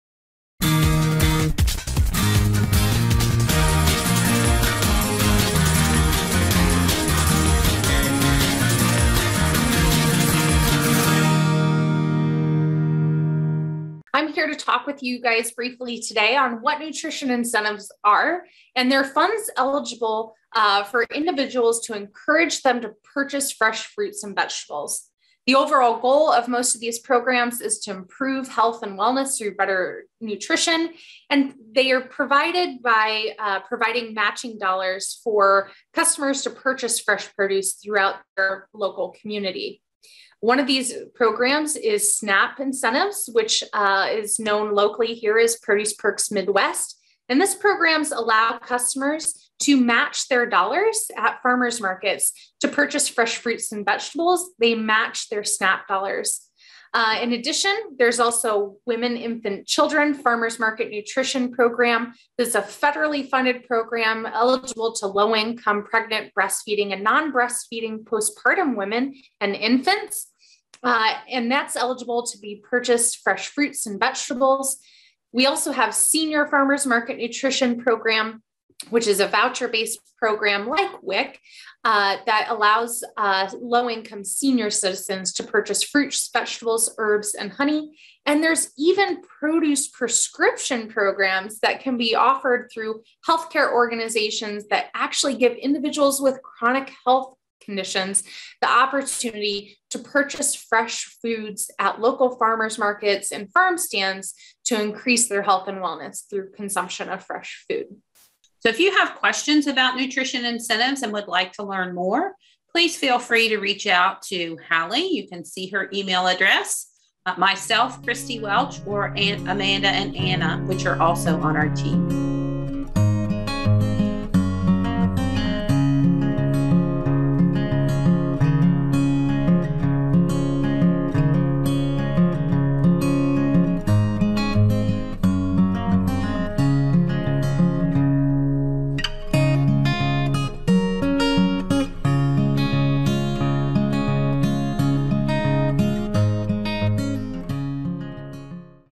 MARKETING MATTERS with Christie Welch - Nutrition Incentives.
MARKETING MATTERS with Christie Welch - Nutrition Incentives.   In this short video, Christie talks with Hallie Williams, -  Agriculture and Natural Resouces Educator at The Ohio State University Extension (CFAES) in Seneca County about the different incentives individuals can use in acquiring quality food at farmer markets.

Staying Healthy, Safe, and Connected - Ohio Department of Aging Annual Report, Fiscal Year 2020